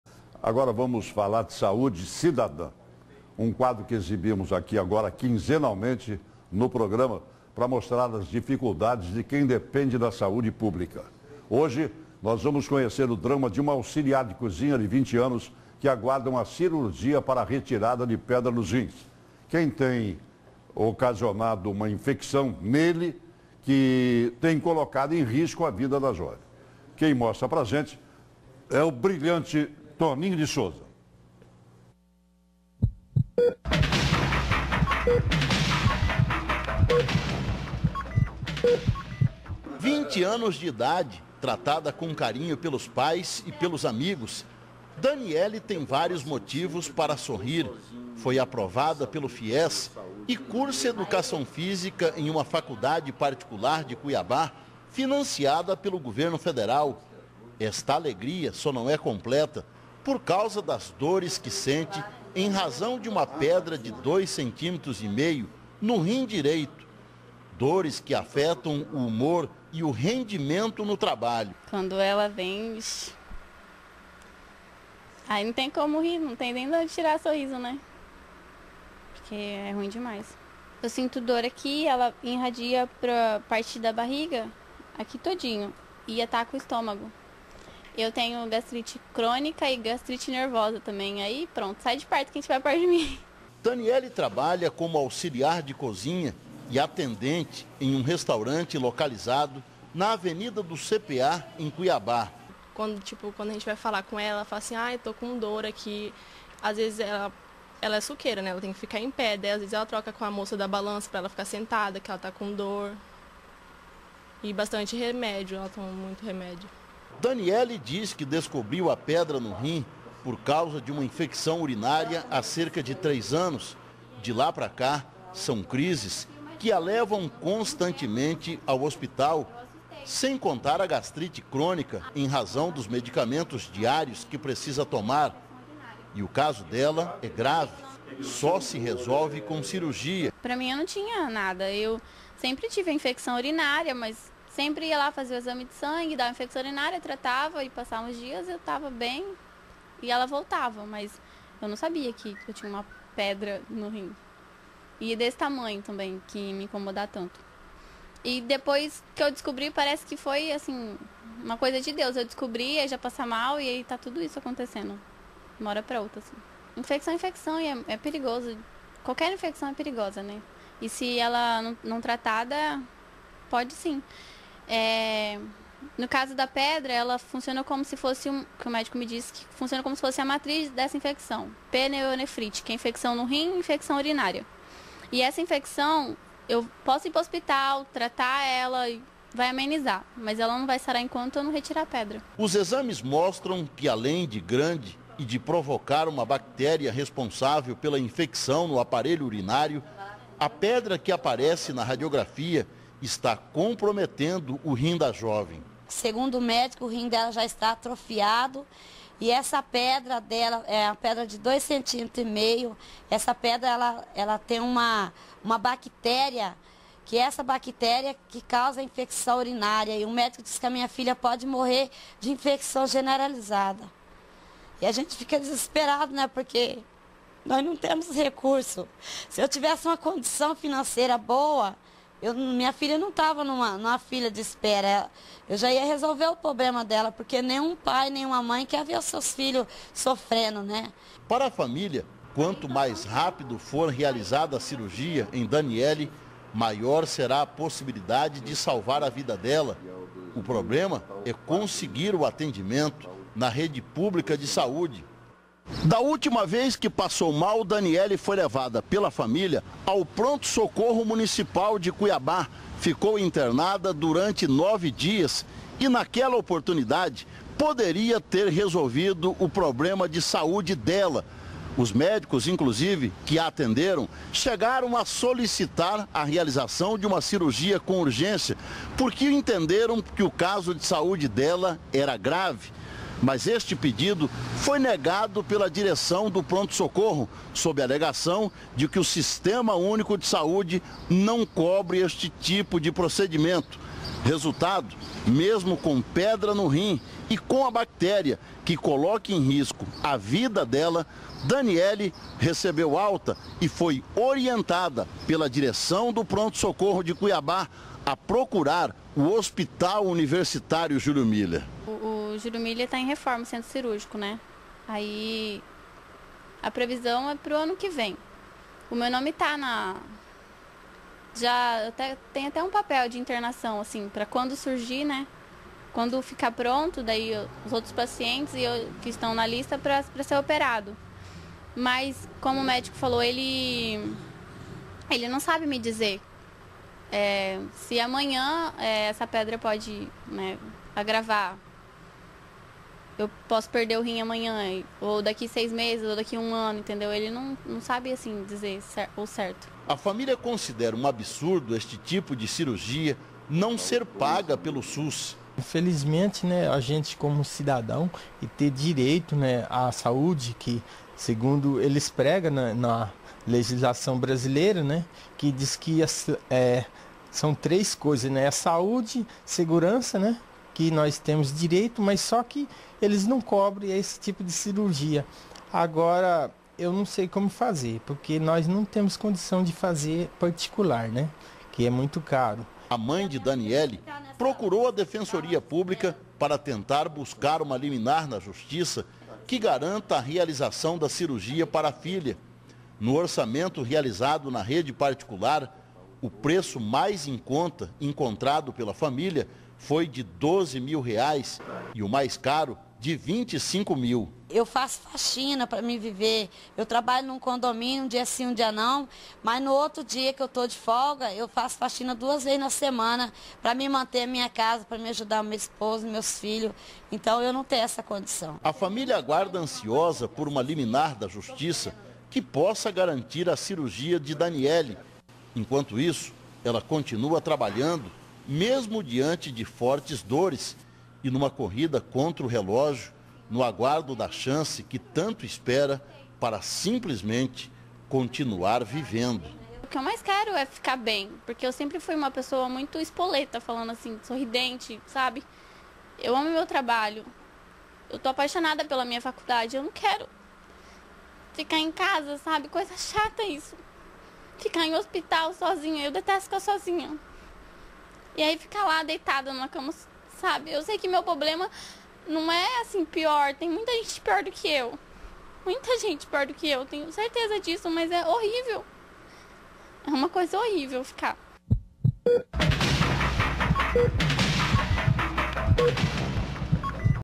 Jovem aguarda por retirada de pedra nos rins
Problema tem ocasionado infecção que coloca a vida da vítima em risco.